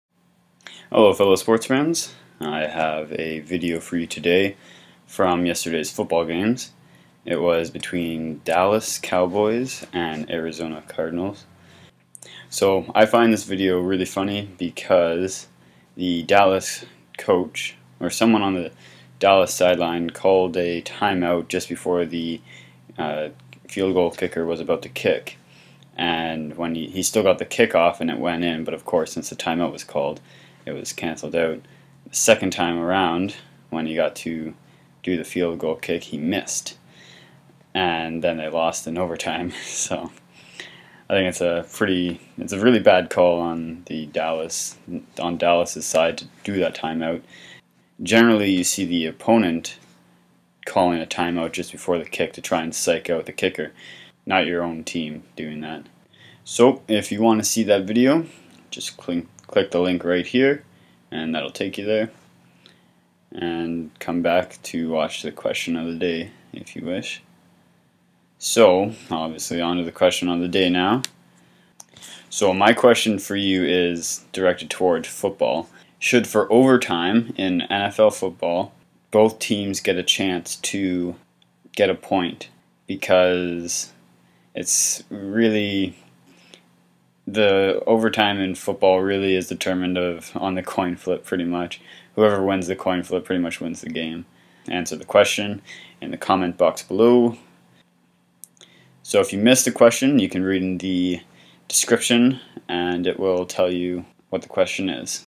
Dallas Cowboys Field Goal Kick Fail!
Dallas Cowboys miss field goal after time out and lose in OT.

Question of the day: In NFL, should OT end after first point or after both teams have a chance to get points?

Leave your answers in the comment box below.